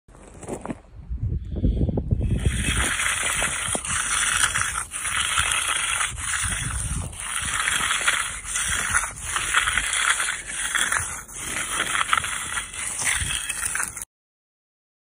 Soothing Sounds of Construction 100 P#shorts
Here is a Video of myself finishing the edge of a Concrete pad.